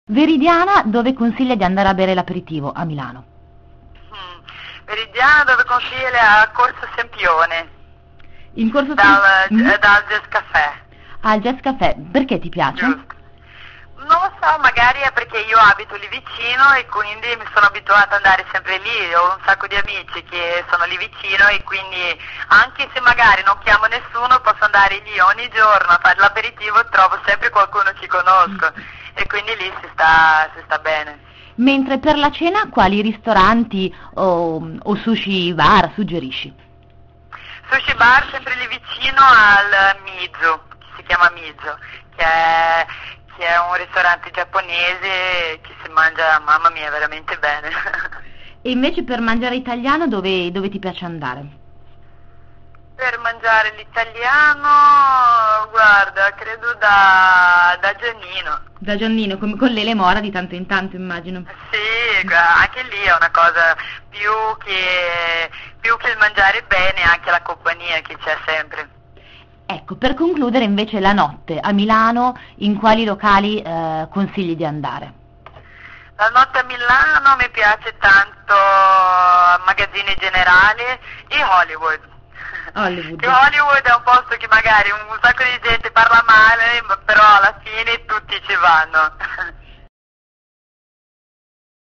Affari di sera/ Veridiana tra sushi bar e discoteche
Da Giannino all'Hollywood: l'ex velina e concorrente dell'Isola dei Famosi svela ad Affaritaliani.it i locali che frequenta a Milano